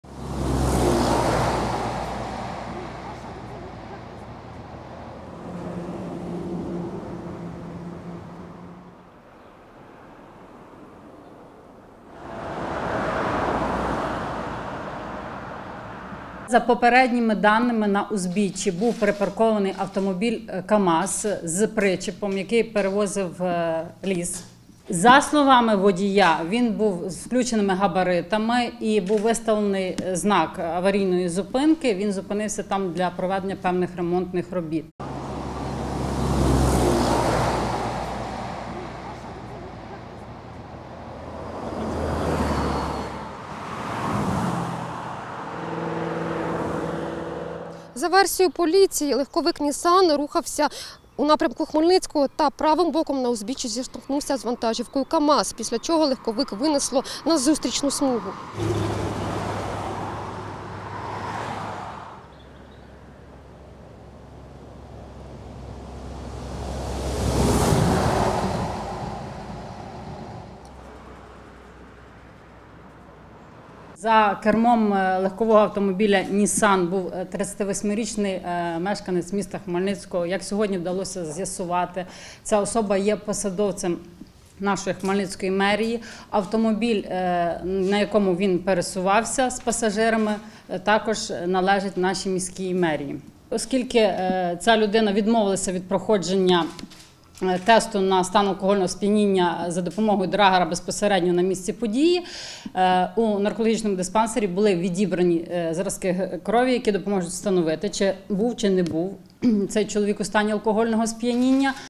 Роману Миколаїву сьогодні оголосять про підозру – обласна поліція
В автомобілі, який сьогодні близько 12-ої ночі на в’їзді до Хмельницького врізався у лісовоз, їхало семеро людей. Двоє з них загинули – це чоловік і жінка. П’ятеро, серед яких троє – діти, отримали травми, повідомила прес-секретар обласного управління поліції Інна Глега. За кермом авто, нагадаю, був директор Департаменту освіти і науки Хмельницької міської ради Роман Миколаїв. Цю інформацію нам підтвЕрдив міський голова Олександр Симчишин. У ДТП Роман Миколаїв отримав травми, але після надання медичної допомоги госпіталізації не потребував, сказав заступник головного лікаря Хмельницької міської лікарні Михайло Соболь. Зараз Роман Миколаїв перебуває в ізоляторі тимчасового тримання. Сьогодні, за словами Інни Глеги, йому мають оголосити про підозру у вчиненні правопорушення, передбаченого частиною 3 статті 286 Кримінального кодексу України. За словами Інни Глеги, чиновник відмовився давати будь-які покази у якості підозрюваного. Коли ж його допитували як свідка, сказав, що не пам’ятає, як сідав за кермо.